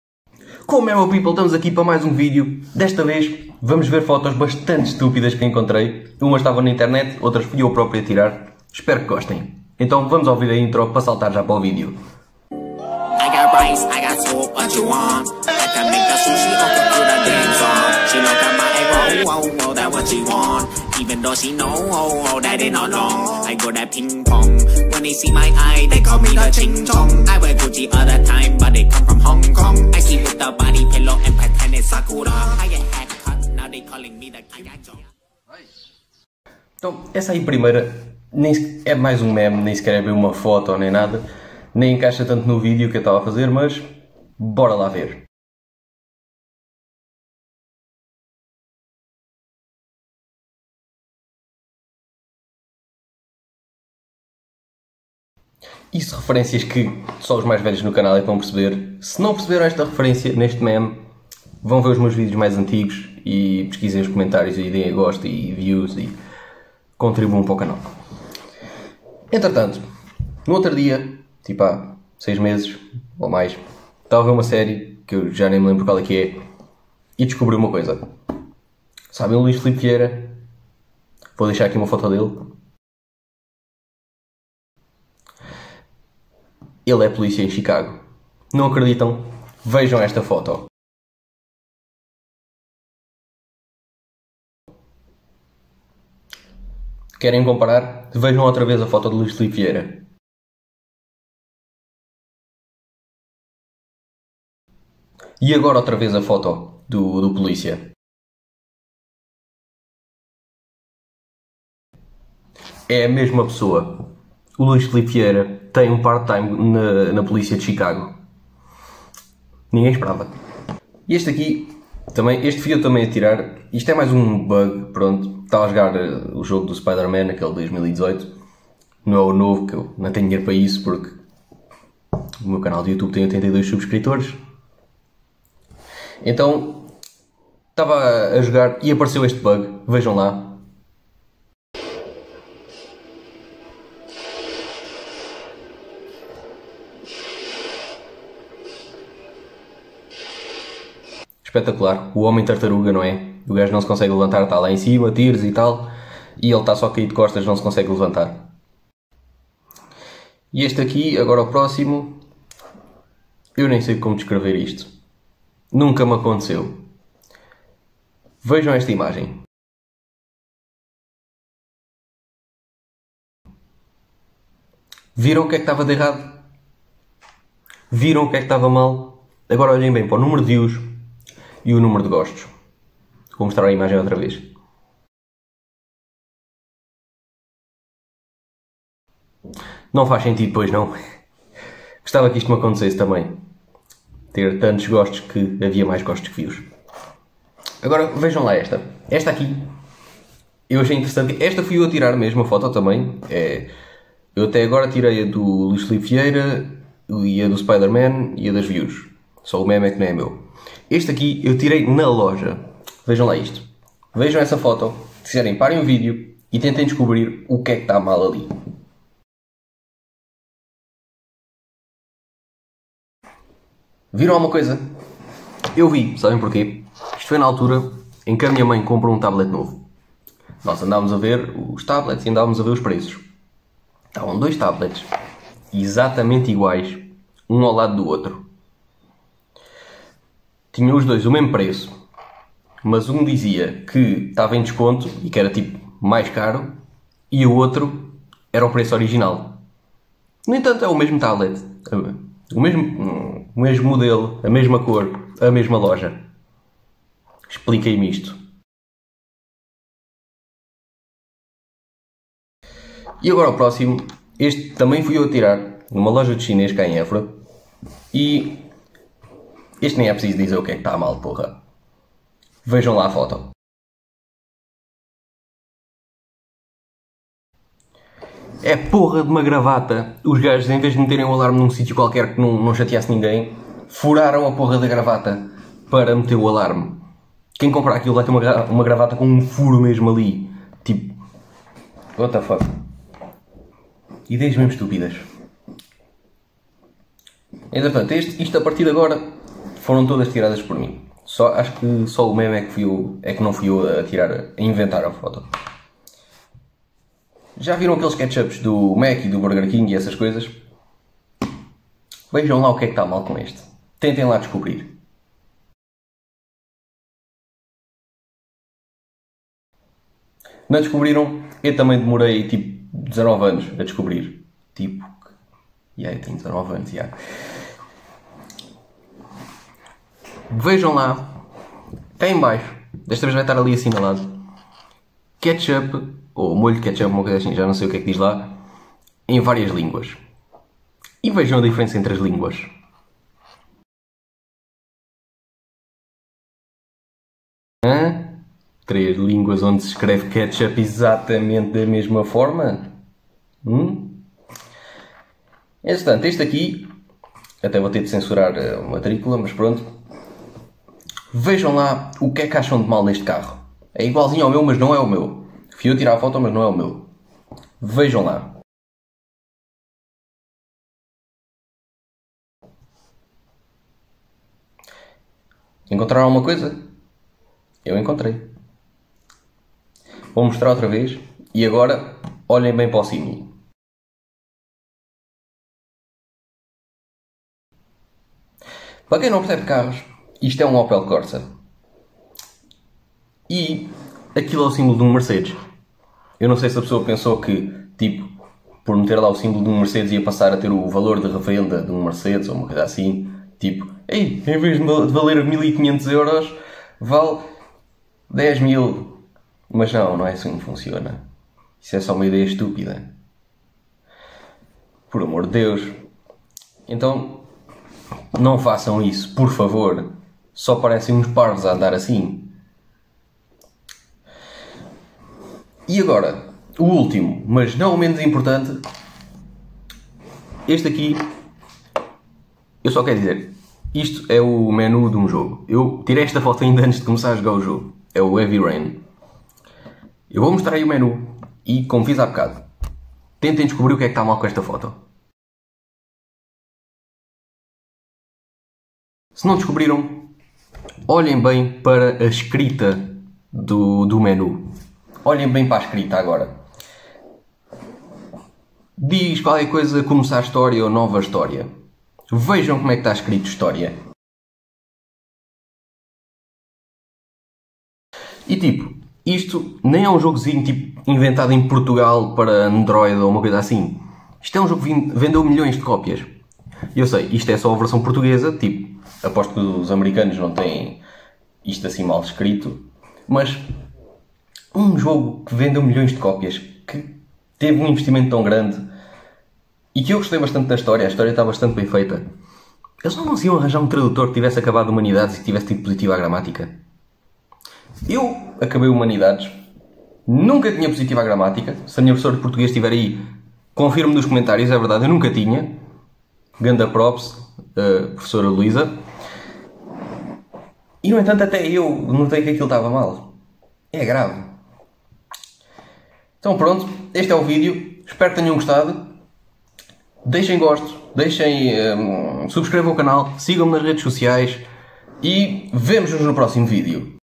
As imagens mais ESTUPIDAS de sempre!!!!
Desta vez trago algumas imagens que eu encontrei e que na minha opinião são muito á toa.